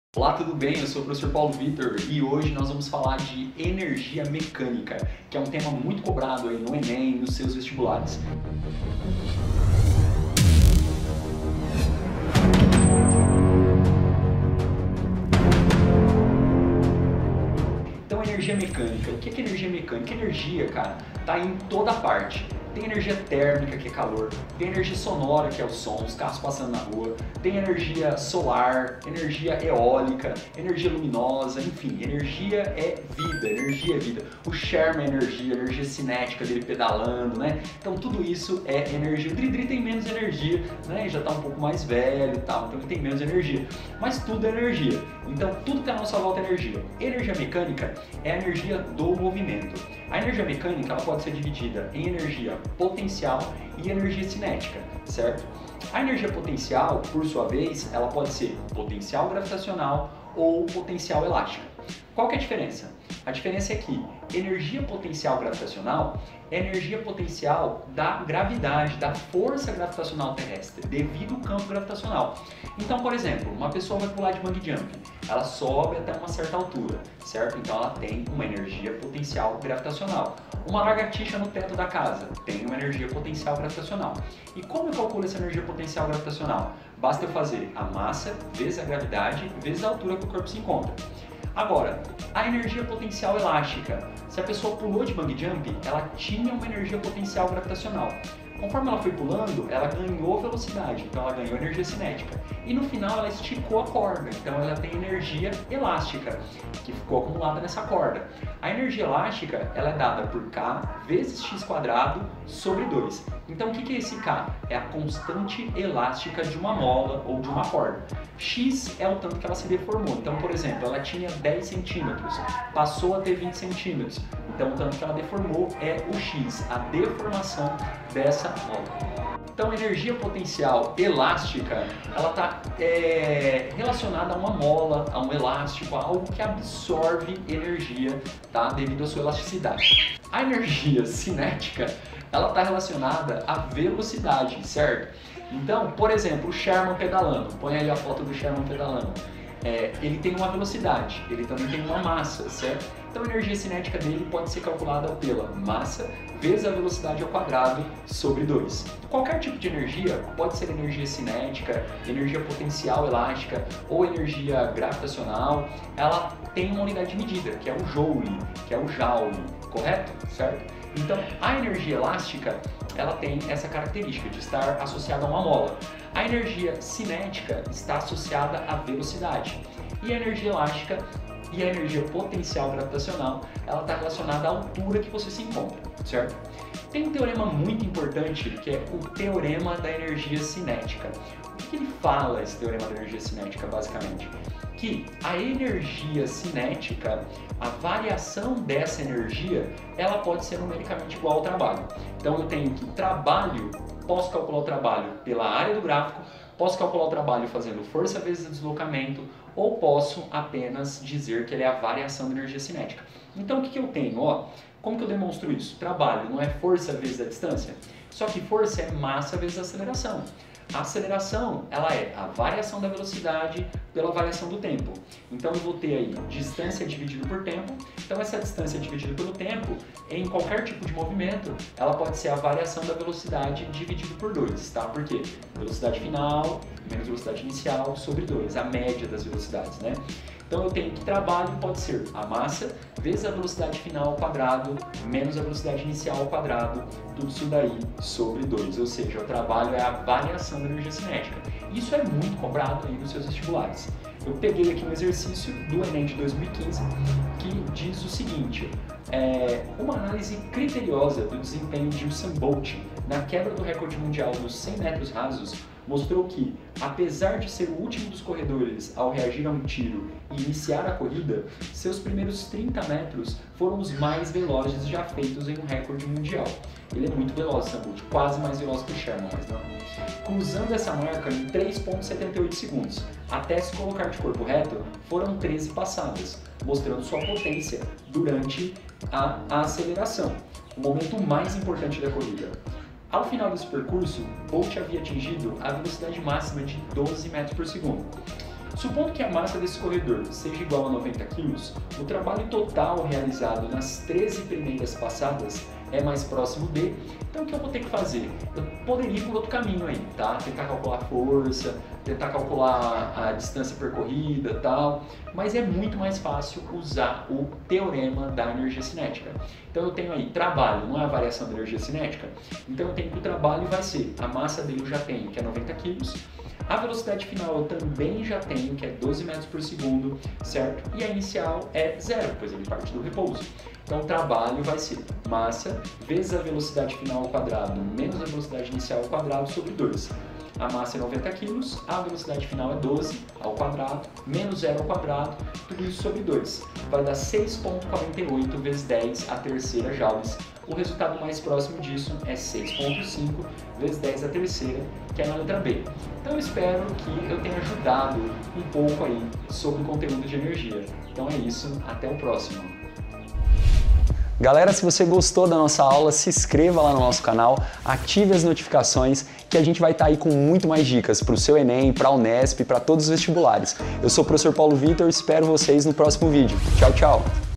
Trabalho e energia
Estudo da Energia Mecânica, energia potencial gravitacional, energia cinética, e teorema do Trabalho e Energia Cinética.